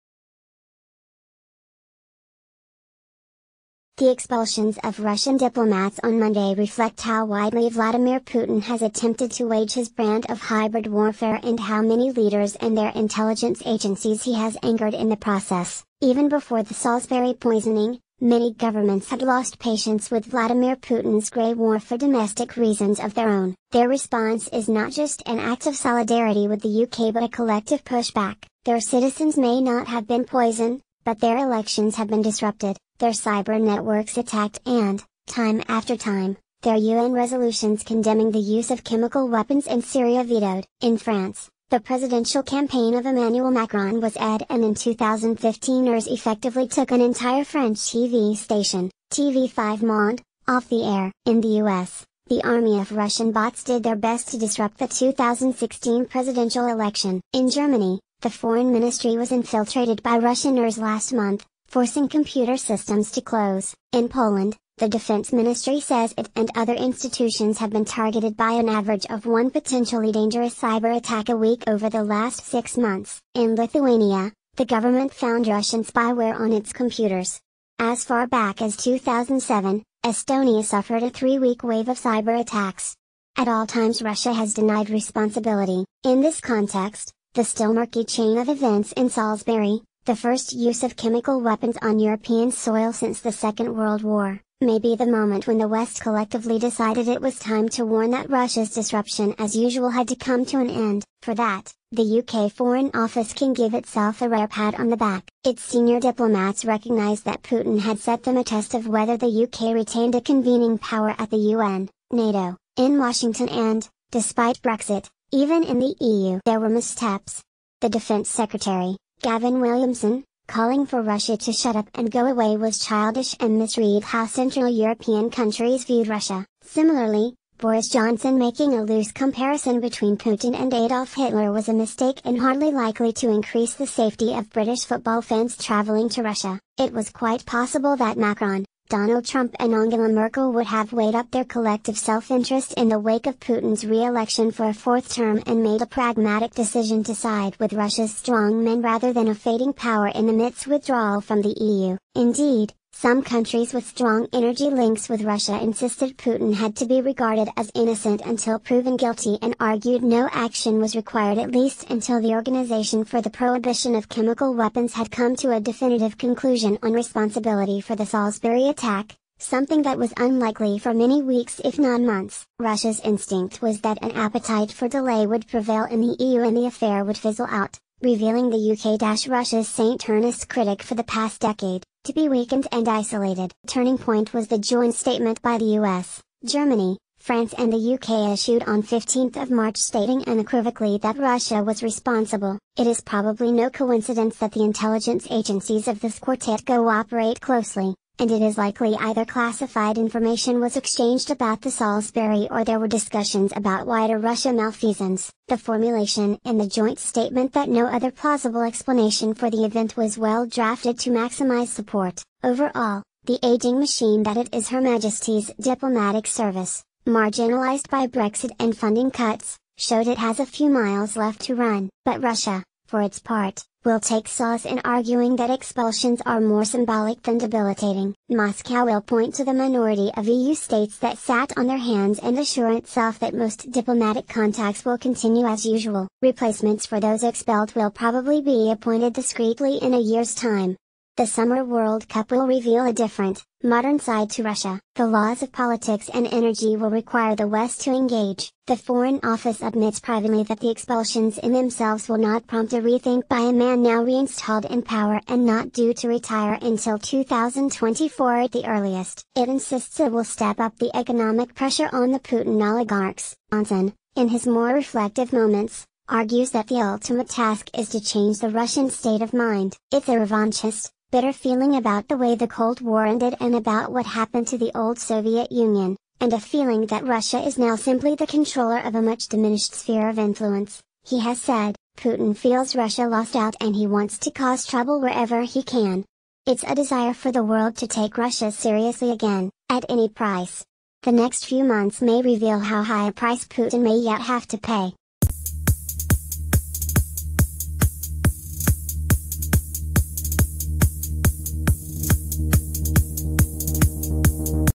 Expulsions of Russians are pushback against Putin's hybrid warfare
The expulsions of Russian dipl...